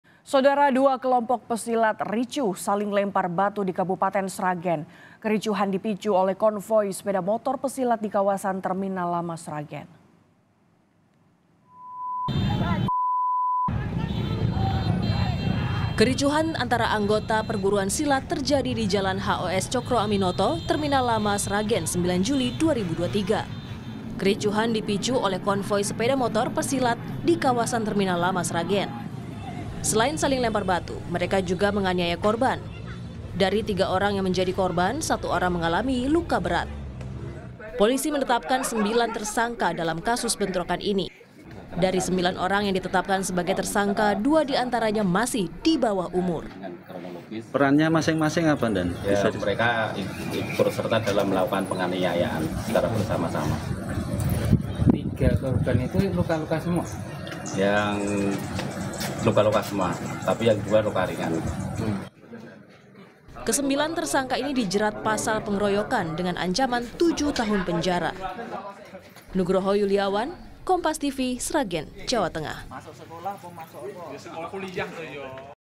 Sembilan Tersangka Bentrokan antar Perguruan Silat di Sragen Terancam 7 Tahun Penjara!
SRAGEN, KOMPAS.TV - Kericuhan antar anggota perguruan silat terjadi di Jalan Hos Cokroaminoto, Terminal Lama, Sragen, 9 Juli 2023.

Kericuhan dipicu oleh konvoi sepeda motor pesilat di kawasan Terminal Lama Sragen.

Selain saling lempar batu, mereka juga menganiaya korban.

Dari 3 orang yang menjadi korban, satu orang mengalami luka berat.

Baca Juga Bentrokan Perguruan Silat di Sragen, Polisi Berencana Mengundang Pimpinan Perguruan Silat di

Polisi menetapkan 9 tersangka dalam kasus bentrokan ini.

Dari sembilan orang yang ditetapkan sebagai tersangka, dua diantaranya masih dibawah umur.

Kesembilan tersangka ini dijerat pasal pengeroyokan, dengan ancaman 7 tahun penjara.